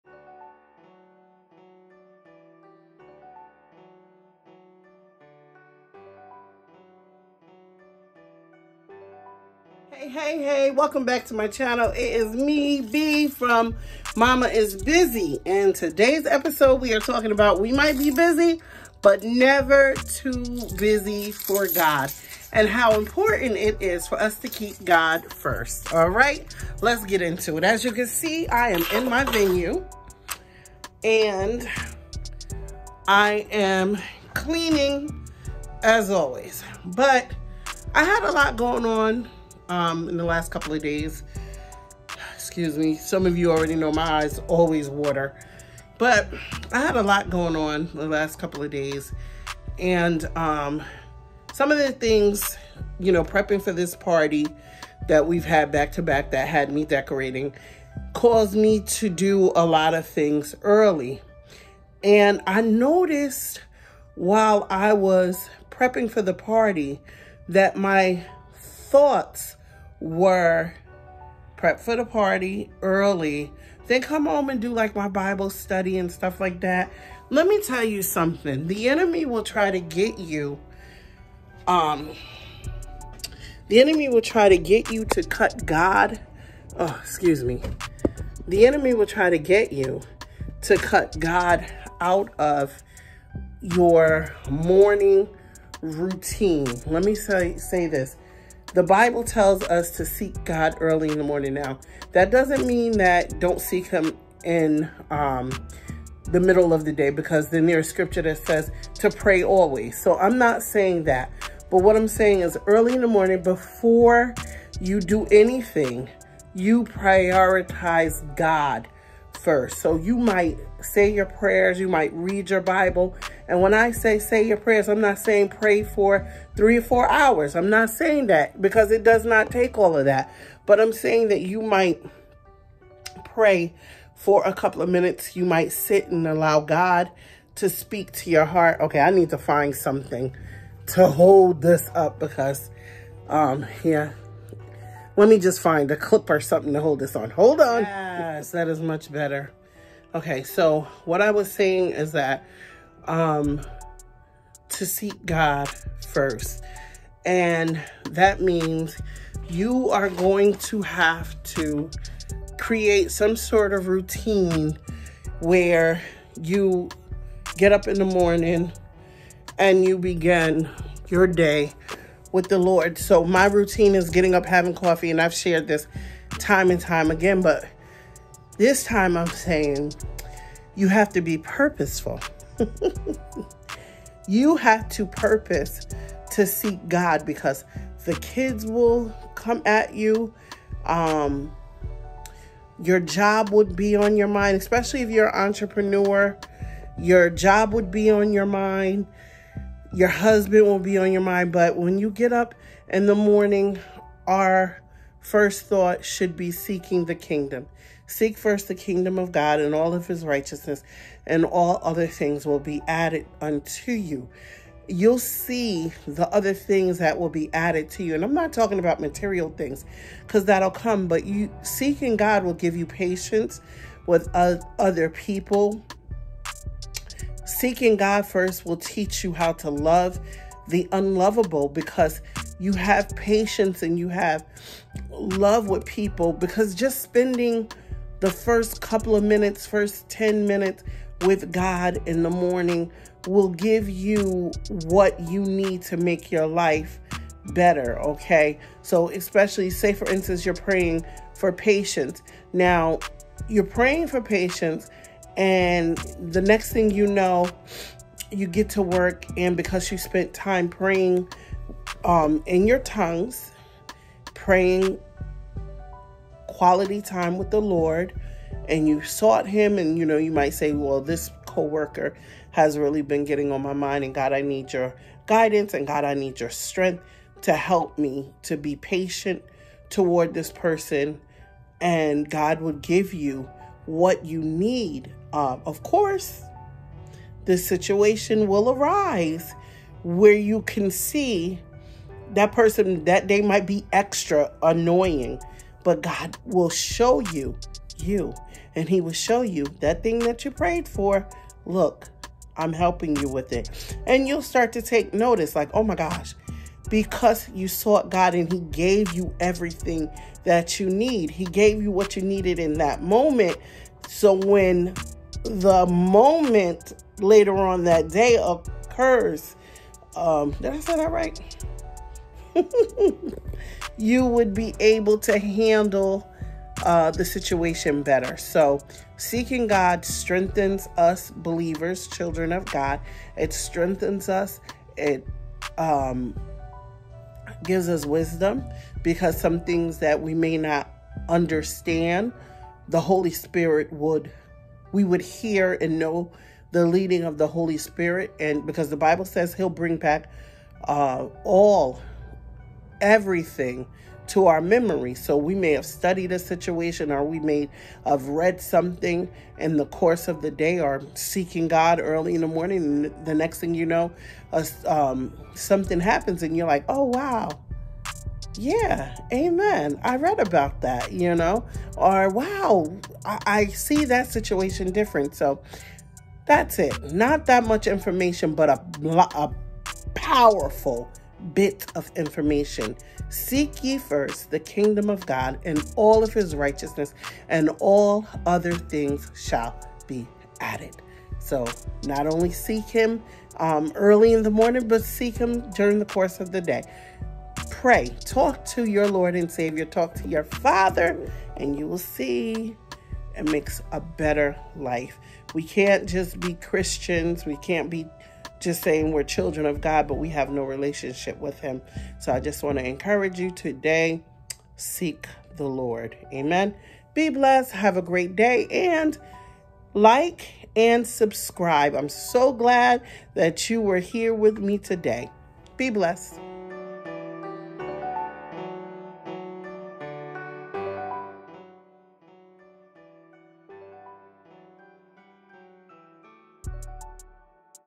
Seek God First
This episode is all about seeking god first. It’s important that we seek him early in the morning. It’s also important that we seek him before any minor or major decisions. God wants to be in relationship with us. Seeking him let’s him know we need his direction Matthew 6:33 tells us to seek first the kingdom of God and all other things will be added.